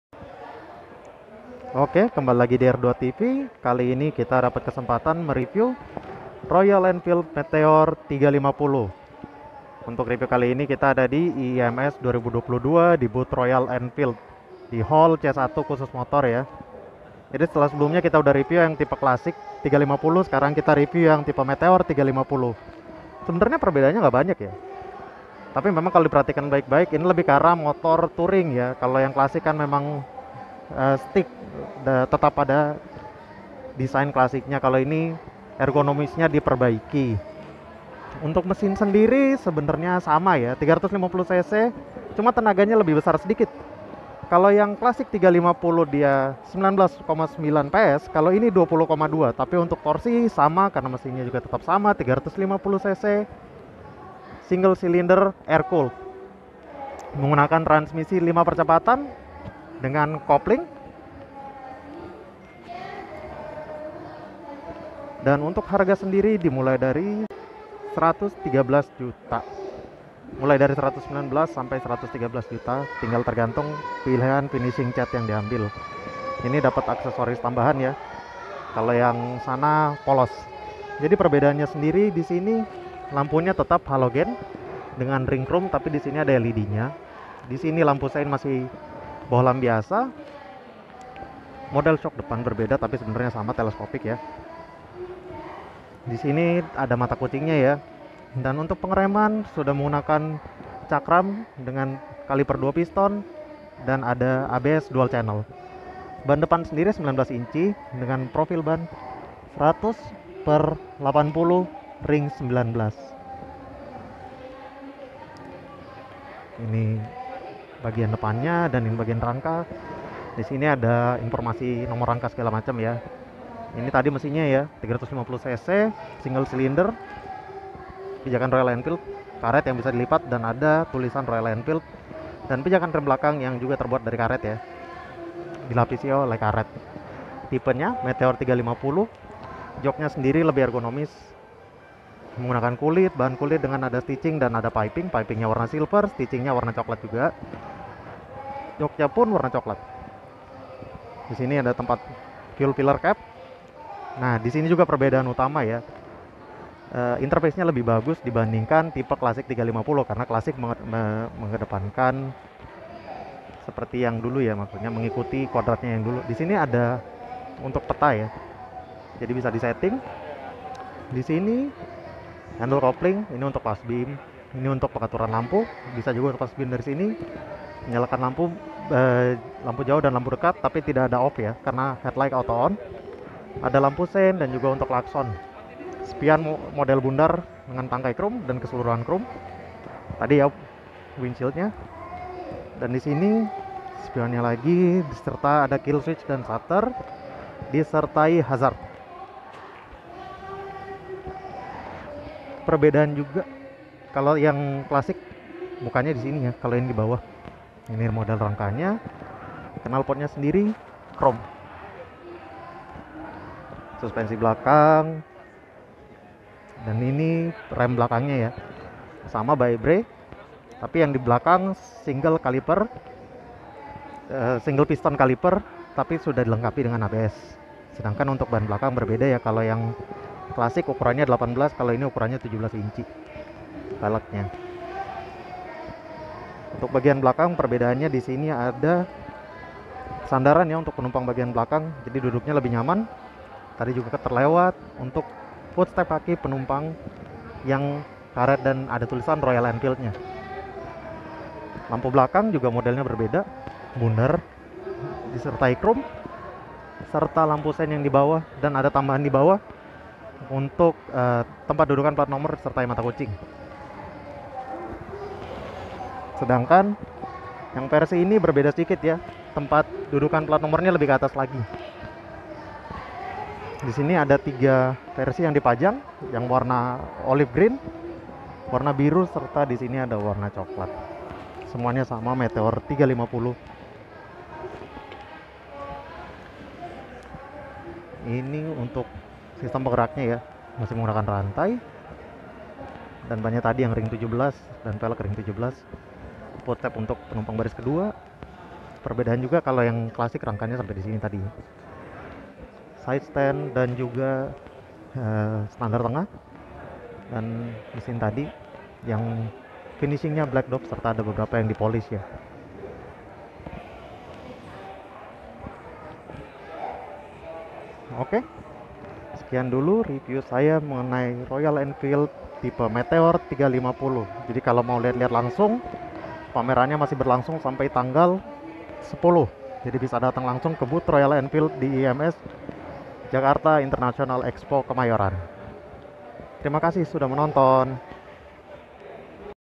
2022 Royal Enfield Meteor 350
Pameran IIMS Hybrid 2022 Berlangsung Mulai 31 Maret 2022 Hingga 10 April 2022 Dan Booth Royal Enfield Terletak Di Hall C1, Jakarta International Expo Kemayoran.